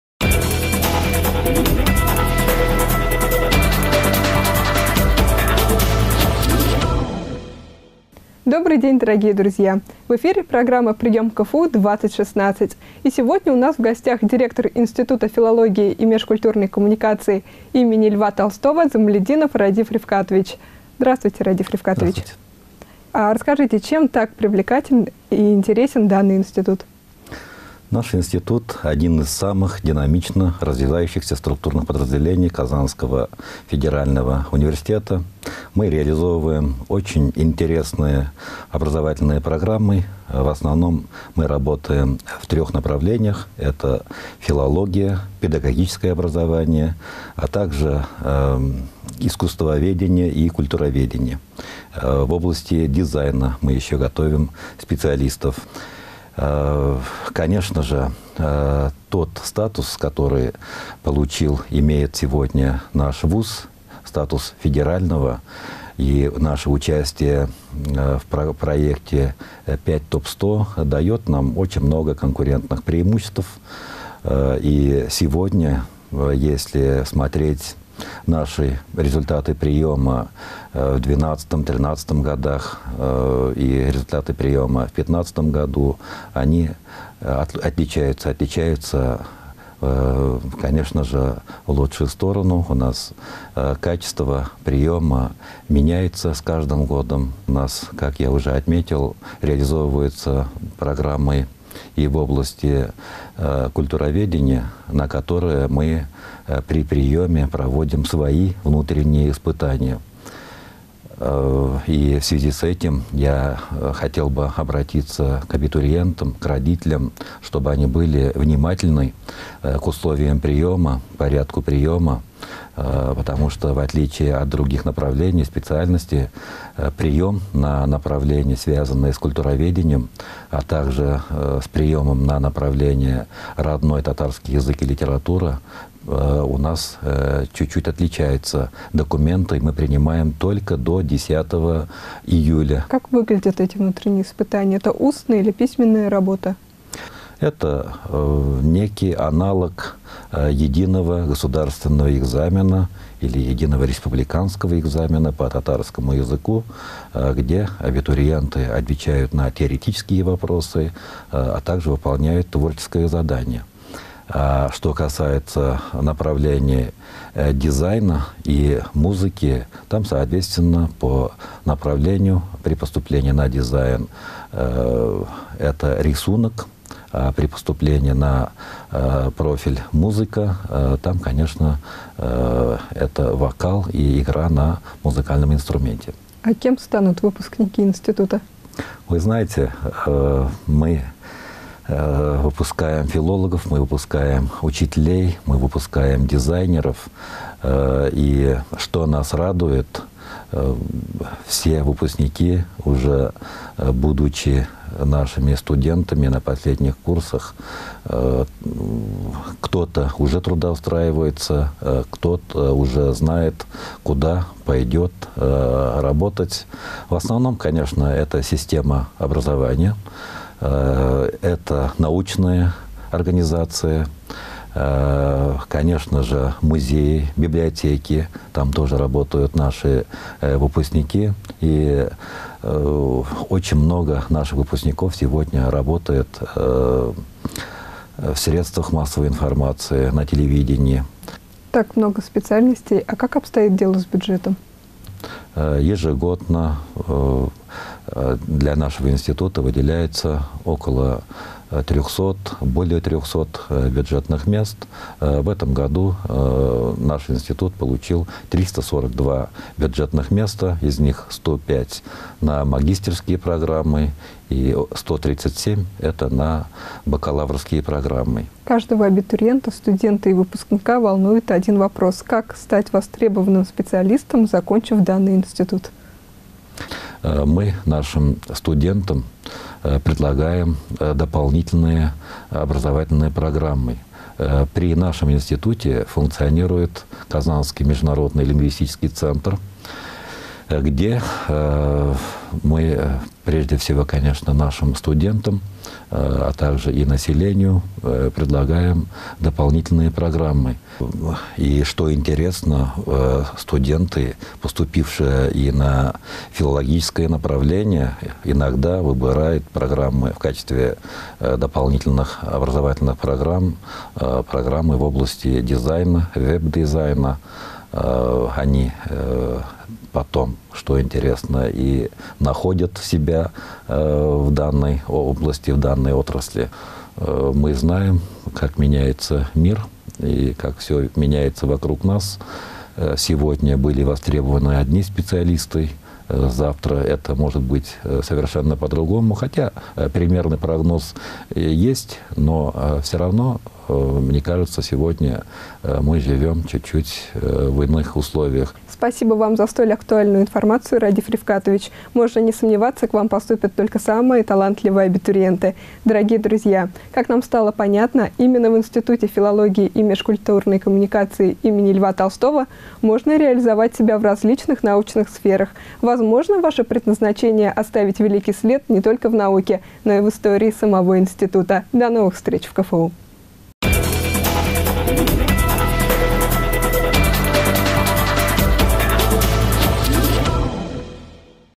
Прием КФУ-2016. Директор ИФМК им. Льва Толстого Радиф Замалетдинов
История Института филологии и межкультурной коммуникации  КФУ началась в 1804 году со дня открытия отделения словесных наук Казанского императорского университета. Названный в честь Льва Толстого, бывшего студента Казанского университета, сегодня ИФМК является одним из крупнейших институтов Казанского федерального университета. Институт готовит высококвалифицированных специалистов в области филологии, искусств и межкультурной коммуникации. Выпускники ИФМК работают в разнообразных сферах: научной, образовательной, в области массовой информации -  телевидение, газеты, радио, пресс-службы, рекламные агентства и PR-агентства, а также в органах государственного управления и коммерческих организациях.